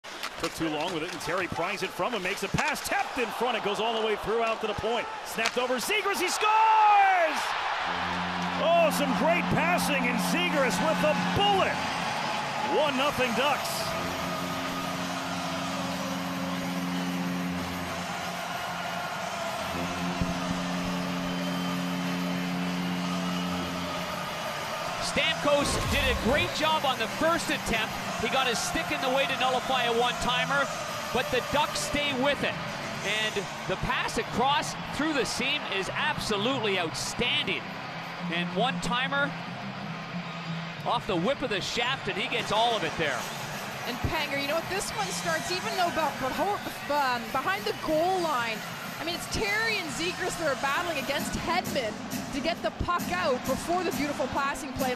Trevor Zegras with a laser 😎
Trevor Zegras slaps the puck past goaltender Brian Elliott to give the Ducks a 1-0 lead.